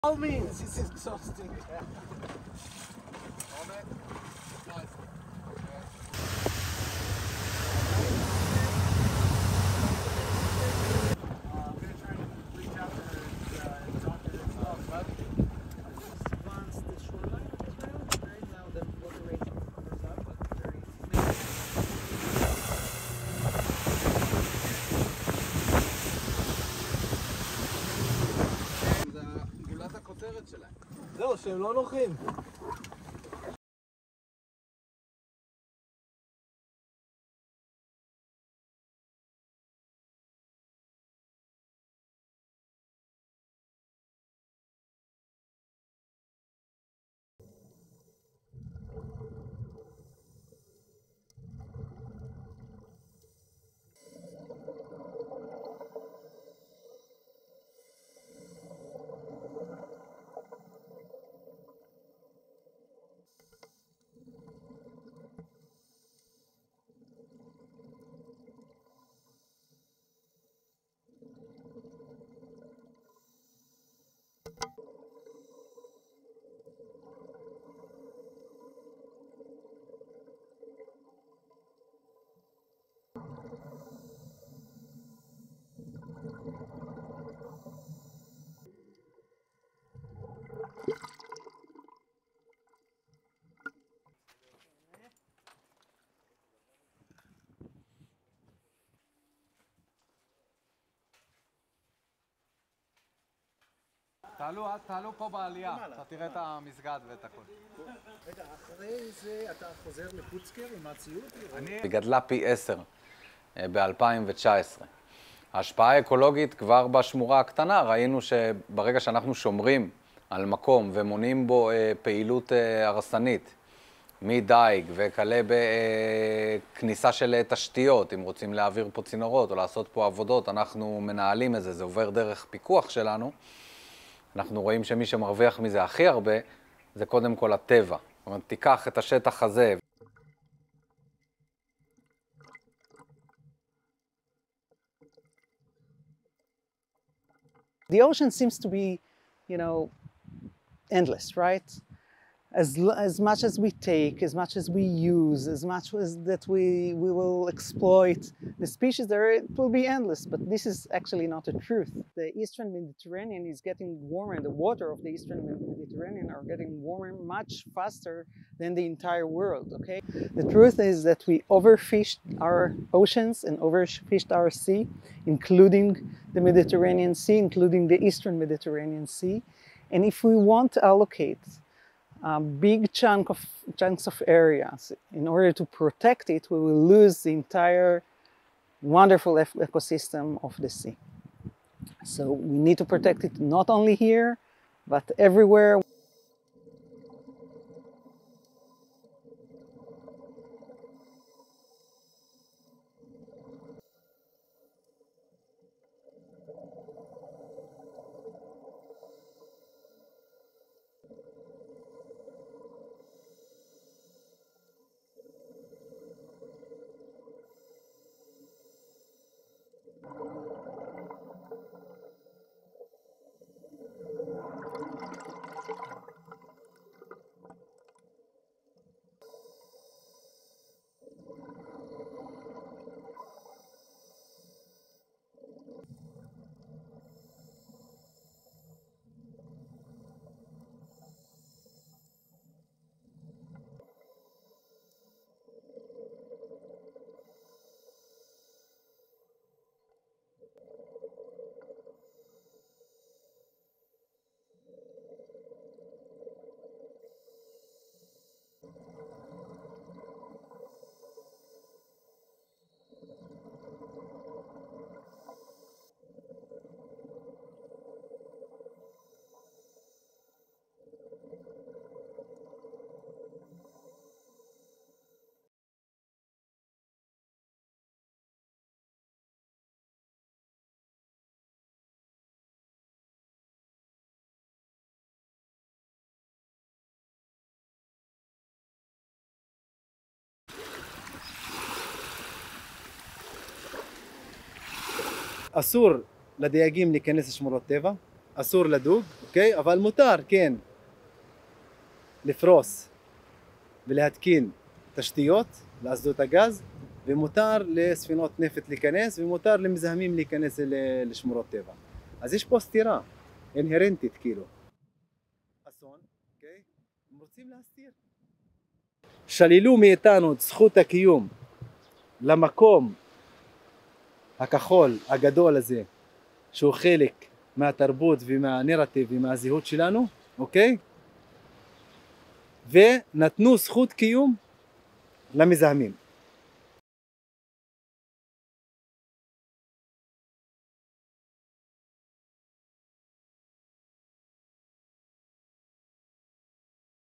Israel expands nature reserve into Mediterranean
RESTRICTION SUMMARY:
ASSOCIATED PRESS
Akhziv National Park - 26 October 2022
1. Various of Nature and Parks Authority (NPA) staff steering the boat and preparing to dive
2. Close of digital map
3. Close of marine ranger Eyal Miller coordinating with the Israeli military
4. Close of marine biologist Ruthy Yahel testing her air supply
5. Various aerials of submerged rocks along the shore ++MUTE++
6. Various of Miller and Yahel diving
7. Tracking shot of ray swimming at around 30 meters (98 feet) depth
8. Close of algae
9. Mid of reef
10. Close of Miller surfacing
11. Various of Miller walking into the boat shed at the NPA facility in Akhziv
12. SOUNDBITE (Hebrew) Eyal Miller, Nature and Parks Authority marine ranger:
"This nature reserve grew tenfold in 2019. The ecological impact, even in the smaller reserve, we saw that from the moment that we protect a place and prevent destructive activity, from fishing to the entry of infrastructure — if they want to pass pipelines through or do work, we manage it, it's done with our oversight — we see that the main beneficiary is nature."
13. Mid of Yahel at the surface
14. SOUNDBITE (English) Ruthy Yahel, Nature and Parks Authority marine biologist:
"The ocean seems to be, you know, endless right? As much as we take, as much as we use, as much as we exploit the species, it will be endless. But this is actually not the truth."
15. Mid of Yahel UPSOUND (English): "The Eastern Mediterranean is getting warmer much faster than the entire world."
16. SOUNDBITE (English) Ruthy Yahel, Nature and Parks Authority marine biologist:
"The truth is that we overfished our oceans our oceans and overfished our sea, including the Mediterranean Sea, including the Eastern Mediterranean Sea, and if we won't allocate a big chunk of areas in order to protect it, we will lose the entire wonderful ecosystem of the sea. So we need to protect it, not only here but everywhere."
17. Various of Yahel and Miller diving
18. Tracking shot of reef
19. Various of Yahel and Miller diving
20. Various of fish
21. Close of algae
22. Mid of algae and sea slug
ASSOCIATED PRESS
Jisr Al-Zarqa - 3 November 2022
23. Aerial of the fishing village of Jisr Al-Zarqa ++MUTE++
24. Mid of fishing boats ++MUTE++
25. Mid of fishermen repairing a net ++MUTE++
26. Mid of a boat next to a harbor tower
27. SOUNDBITE (Hebrew) Sami Ali, spokesman for the Israel Fisherman's Union:
"It's forbidden for fisherman to enter marine reserves, it's forbidden to fish, but it's allowed to lay infrastructure for the gas rigs and for oil ships and polluters to enter the marine reserves. There's an inherent contradiction here."
28. Mid of Ali
29. SOUNDBITE (Hebrew) Sami Ali, spokesman for the Israel Fisherman's Union:
"They stole our right to existence in this big blue place that is part of our tradition, narrative and identity, ok? and gave a right of existence to polluters."
30. Aerial of the fishing village ++MUTE++
31. Aerial of man fishing ++MUTE++
STORYLINE:
Between the cliffs and crags of Israel's submerged prehistoric coastline, a Mediterranean ecosystem is surging back to life.
Giant groupers flourish among the rocks, a psychedelic purple nudibranch sea slug clings to an outcrop, and a pair of rays skate along the undisturbed sandy bottom.
Israel is blazing forward with a plan to protect sections of its 118-mile (190 kilometer) coastline, a measure experts say is crucial to maintaining biodiversity and shielding ecosystems from humanity.
Scientists warn that without protection, remaining marine ecosystems will be devastated.